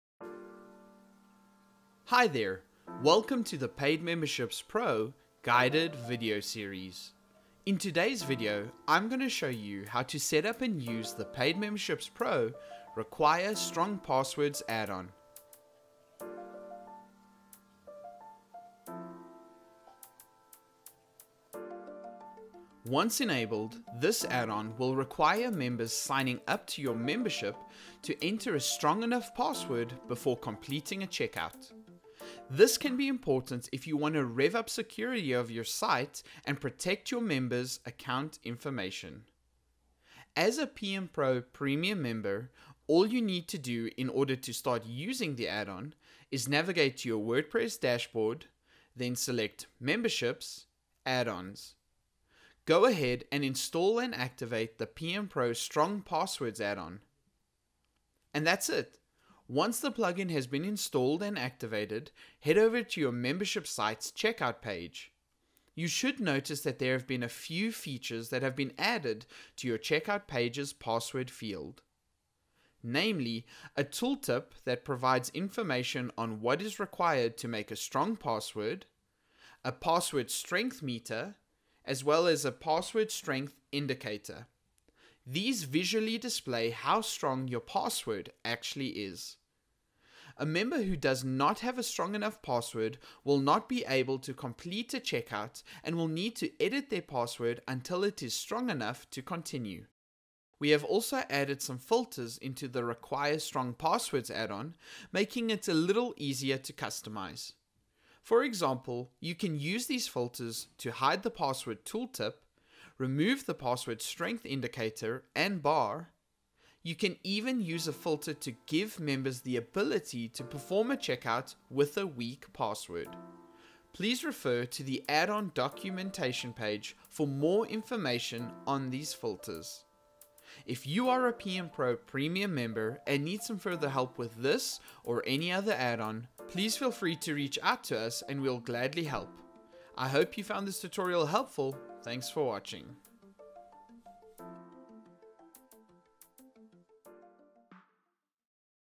Require Strong Passwords Add On | Paid Memberships Pro
Require members to use “Strong” passwords based on required characters.

This Add On checks to make sure the user has submitted a strong password by checking for length, lowercase/uppercase, numbers, special characters, and matching username. The WordPress Password Strength Meter Calculator is also included, allowing members to see how strong their password is. Developers can alter the password requirements by commenting out certain checks as well as adjust or remove the “hint” added to the password field on the checkout page.